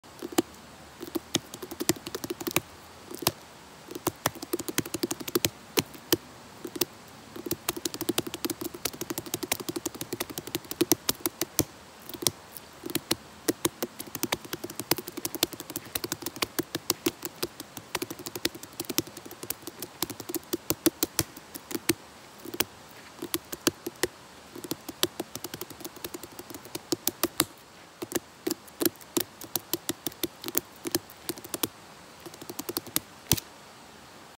冬瓜 【とうがん】叩いてみた
【冬瓜】【叩いてみたら】
どうでしょう？
フレッシュな冬瓜 = トウガホンw
コスパは◎⇒¥250(税込)
が、でっっかいし重い。
賞味期限もあって実用化はムズカシイｨ😅